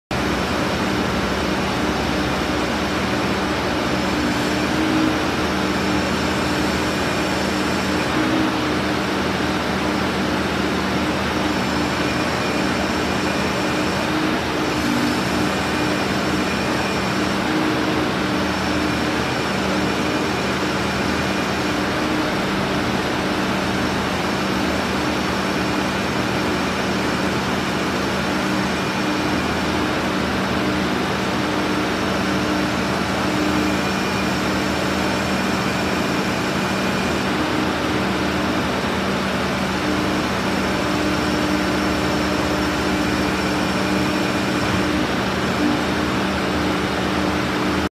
2008 ETI ETC37IH For Sale
Engine: 6.4L V8 Diesel Transmission:  Automatic  A/C 4X4 W/ ETI MODEL:ET037IH 37FT Platform  Height Bucket Unit